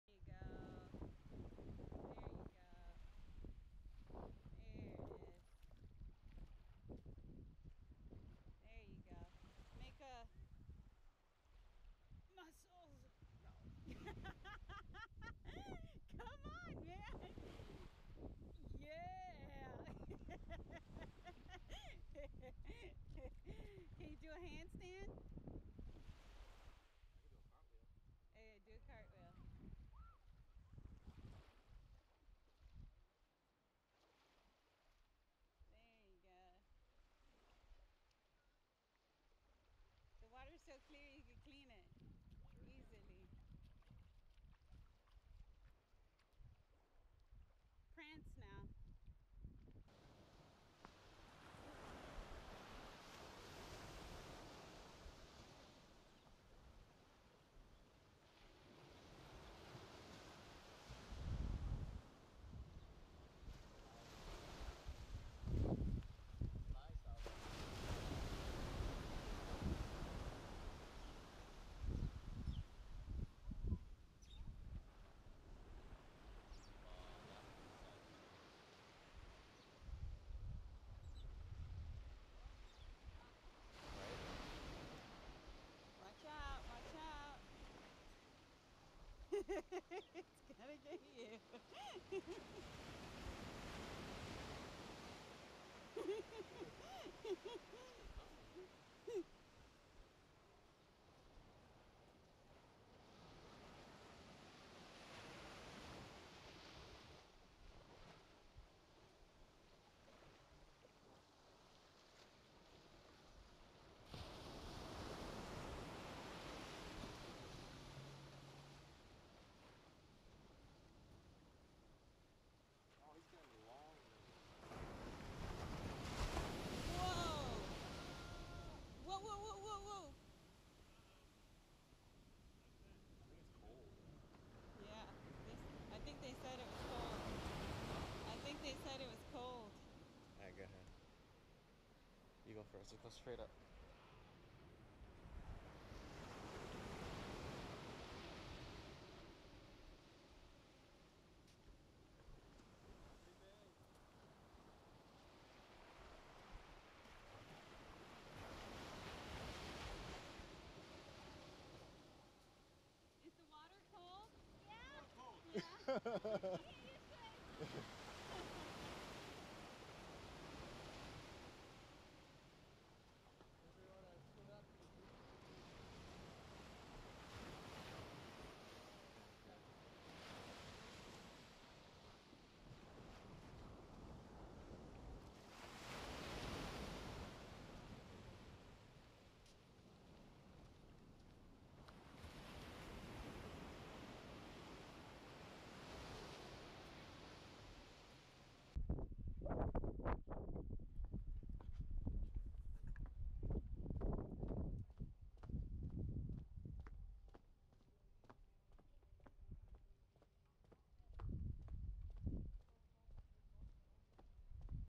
Love Beach I New Providence Island I Nassau Bahamas I St. John I San Juan Puerto Rico
Took a ferry to St. John from St. Thomas and landed in Coco beach. Cloudy day and even got some rain however we made the best of it and the water was warmer than expected and refreshing.